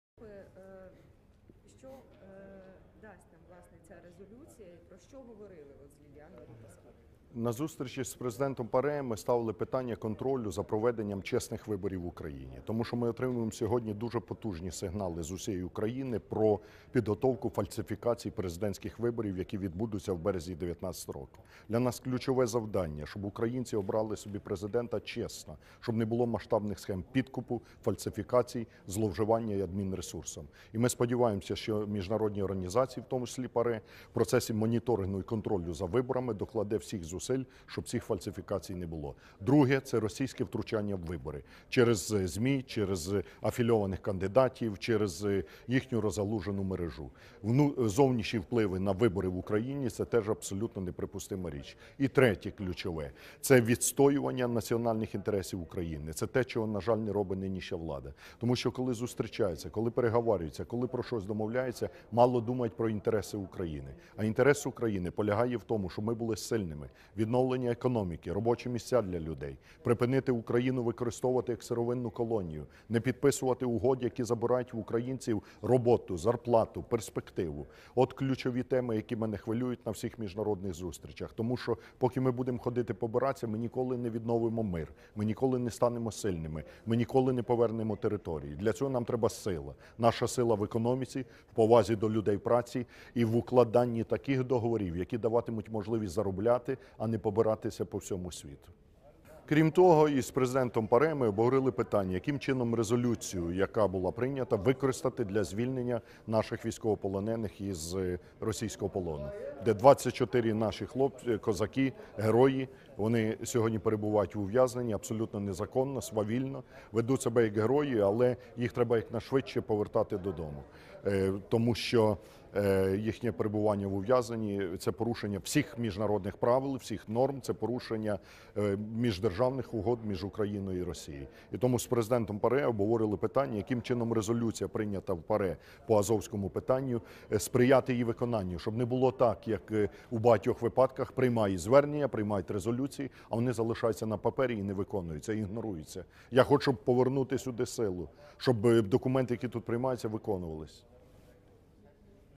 Ляшко у Страсбурзі розповів Президенту ПАРЄ про загрози виборів в Україні
На зустрічі з Президентом ПАРЄ Ліліан Морі-Паск'є лідер Радикальної партії Олег Ляшко розповів про дві головні загрози, які можуть завадити чесному волевиявленню на президентських виборах в Україні. Це масштабні фальсифікації з боку влади та втручання з боку Росії. Щоб завадити цьому, політик закликав Раду Європи та інші міжнародні інституції організувати місії спостерігачів.

«Ми отримуємо дуже потужні сигнали про підготовку фальсифікацій президентських виборів в Україні, які відбудуться в березні 2019 року. Ключове завдання для нас – щоб українці обрали Президента чесно та не було масштабних схем підкупу, фальсифікацій, зловживань адмінресурсом. Ми сподіваємося, що міжнародні організації, в тому числі, ПАРЄ, в процесі моніторингу й контролю за виборами докладе всіх зусиль, щоб цих фальсифікацій не було. Друга загроза – це російське втручання у вибори. Через ЗМІ, прокремлівських кандидатів, розгалужену мережу підкупу. Зовнішні впливи на вибори в Україні – це абсолютно неприпустима річ», - підкреслив Ляшко.

Політик переконаний, щоб ніхто не втручався у справи України, в тому числі, і у виборчий процес, країна має стати сильною.

«Національні інтереси України – це відновлення економіки, робочі місця, високі зарплати. Це припинення використання України як сировинної колонії. Це розірвання угод, які забирають в українців перспективу. Поки ми будемо побиратися, ми ніколи не відновимо мир і не повернемо території. Ми маємо стати сильними. А сила наша в економіці, повазі до людей праці і укладанні таких договорів, які даватимуть можливість заробляти, а не побиратися по всьому світу!», - зауважив лідер РПЛ.

Також Олег Ляшко наголосив на зустрічі, що резолюція щодо ситуації в Азовському морі повинна виконуватися, а не лишатися на папері.

«24 наші хлопці, козаки, герої сьогодні перебувають в ув’язненні абсолютно незаконно. Це порушення всіх міжнародних угод. Їх треба повертати додому. З Президентом ПАРЄ обговорили, яким чином прийнята резолюція сприяти цьому. Щоб не було, як у багатьох випадках – угоди приймаються, а вони залишаються лише на папері. Я хочу повернути силу в цю міжнародну інституцію, аби документи, які тут ухвалюються, виконувались», - підсумував Ляшко.